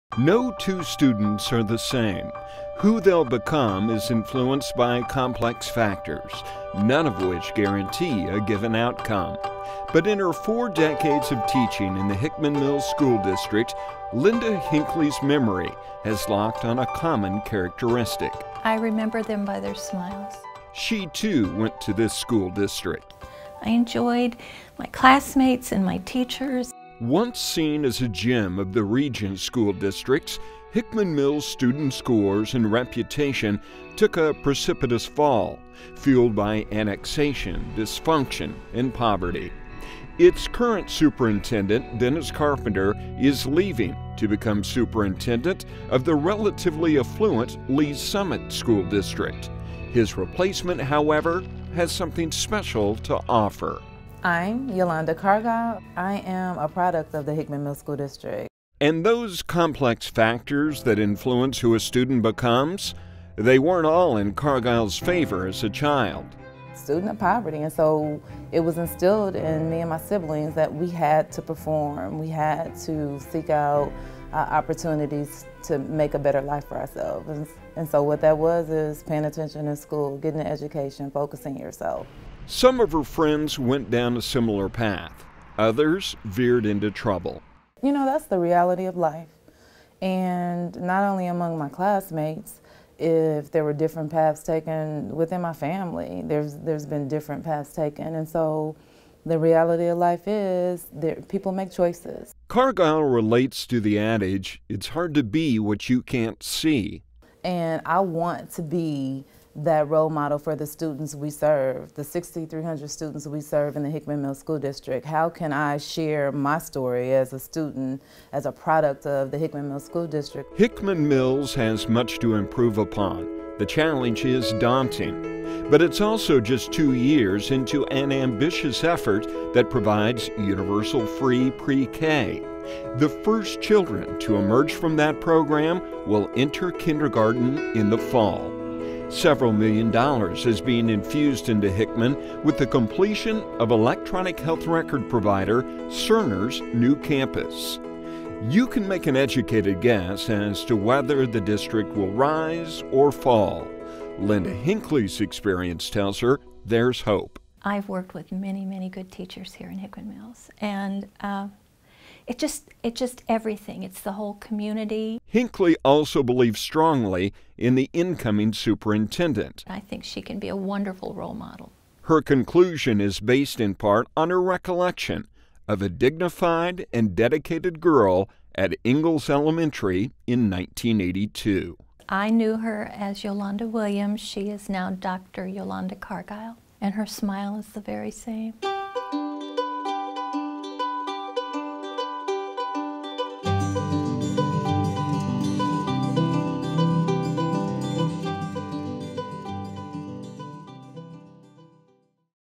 Hickman Hope | Take Note
The Hickman Mills School District in Kansas City, Missouri, is trying to recapture the stability and success it enjoyed decades ago. It's new superintendent, Yolanda Cargile, is a big part of that effort. She grew up poor and wants to serve as a role model for today's students.